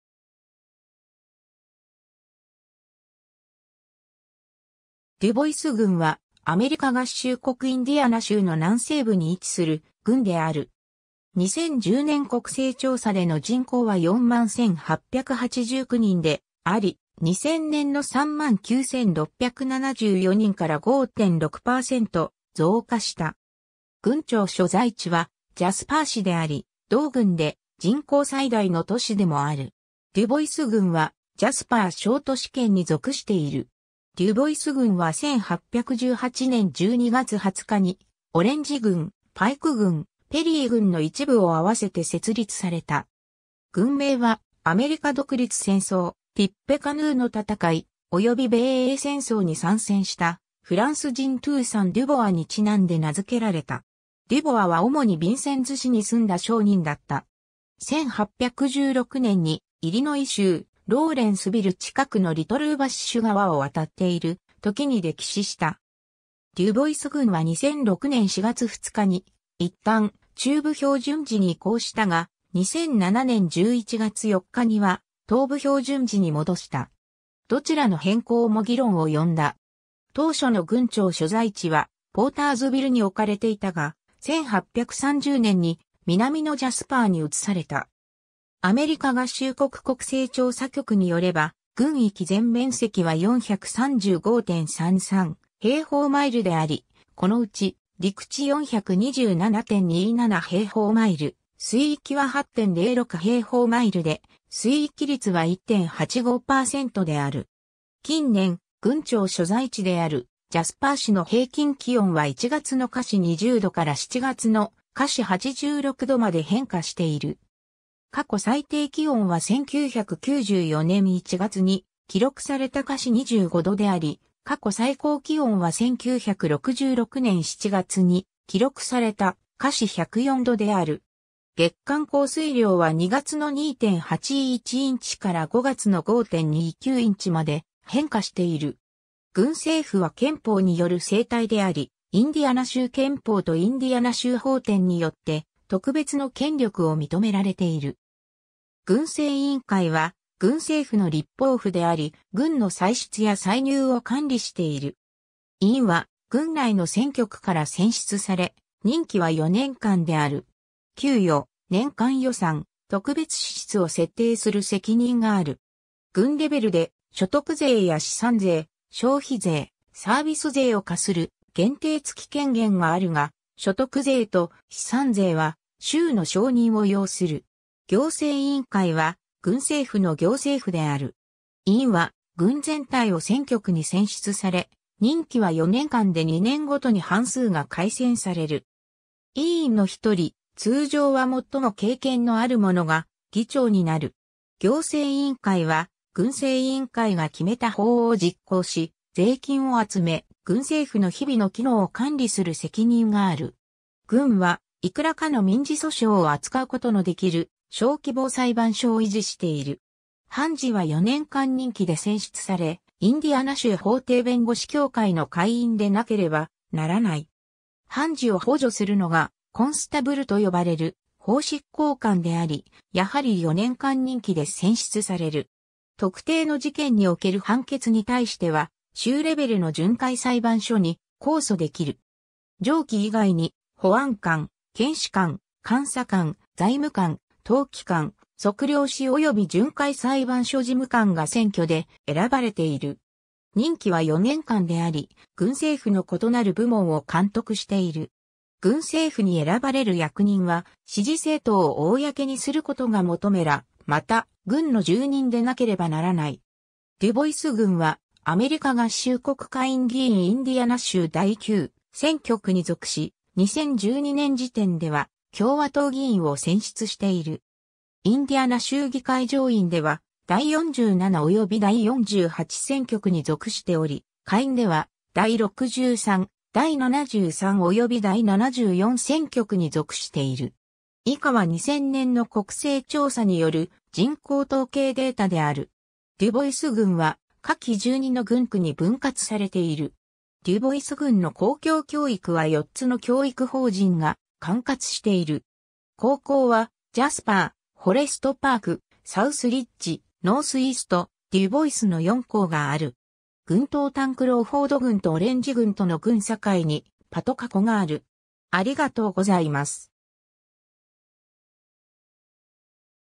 デュボイス郡 (インディアナ州)
デュボイス郡（英: Dubois County）は、アメリカ合衆国インディアナ州の南西部に位置する郡である。
2010年国勢調査での人口は41,889人であり、2000年の39,674人から5.6％増加した。
郡庁所在地はジャスパー市（人口15,038人）であり、同郡で人口最大の都市でもある。
 デュボイス郡はジャスパー小都市圏に属している。
 デュボイス郡は1818年12月20日に、オレンジ郡、パイク郡、ペリー郡の一部を合わせて設立された。
郡名はアメリカ独立戦争、ティッペカヌーの戦い、および米英戦争に参戦したフランス人トゥーサン・デュボアに因んで名付けられた。
デュボアは主にビンセンズ市に住んだ商人だった。
1816年にイリノイ州ローレンスビル近くのリトルウォバッシュ川を渡っているときに溺死した。
 デュボイス郡は2006年4月2日に一旦中部標準時に移行したが、2007年11月4日には東部標準時に戻した。
どちらの変更も議論を呼んだ 。
 当初の郡庁所在地はポーターズビルに置かれていたが、1830年に南のジャスパーに移された。
 近年、郡庁所在地であるジャスパー市の平均気温は1月の20°F (-7 ℃) から7月の86°F (30 ℃) まで変化している。
 郡政府は憲法による政体であり、インディアナ州憲法とインディアナ州法典によって特別の権力を認められている。
 郡政委員会は郡政府の立法府であり、郡の歳出や歳入を管理している。
委員は郡内の選挙区から選出され、任期は4年間である。
給与、年間予算、特別支出を設定する責任がある。
郡レベルで所得税や資産税、消費税、サービス税を課する限定付き権限があるが、所得税と資産税は州の承認を要する。
 行政委員会は郡政府の行政府である。
委員は郡全体を選挙区に選出され、任期は4年間で2年毎に半数が改選される。
委員の一人、通常は最も経験のある者が議長になる。
行政委員会は郡政委員会が決めた法を実行し、税金を集め、郡政府の日々の機能を管理する責任がある。
 郡は幾らかの民事訴訟を扱うことのできる小規模裁判所を維持している。
判事は4年間任期で選出され、インディアナ州法廷弁護士協会の会員でなければならない。
判事を補助するのがコンスタブルと呼ばれる法執行官であり、やはり4年間任期で選出される。
特定の事件における判決に対しては、州レベルの巡回裁判所に控訴できる。
 上記以外に、保安官、検視官、監査官、財務官、登記官、測量師および巡回裁判所事務官が選挙で選ばれている。
任期は4年間であり、郡政府の異なる部門を監督している。
郡政府に選ばれる役人は支持政党を公にすることが求めら、また郡の住人でなければならない。
 デュボイス郡はアメリカ合衆国下院議員インディアナ州第9選挙区に属し、2012年時点では共和党議員を選出している。
インディアナ州議会上院では第47および第48選挙区に属しており、下院では第63、第73および第74選挙区に属している。
 デュボイス郡は下記12の郡区に分割されている。
 デュボイス郡の公共教育は4つの教育法人が管轄している。
 高校は、ジャスパー、フォレストパーク、サウスリッジ、ノースイースト・デュボイスの4校がある。
 郡東端クロウフォード郡とオレンジ郡との郡境にパトカ湖がある。